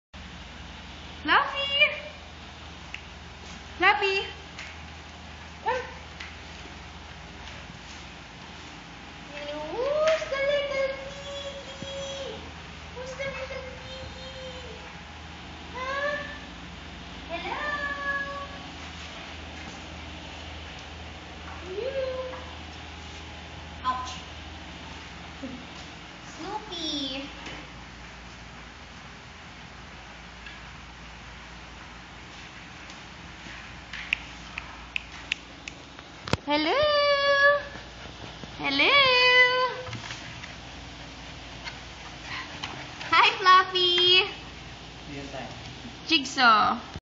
old english sheepdog puppies
3male 3 female old english sheepdog date of birth march 4 2014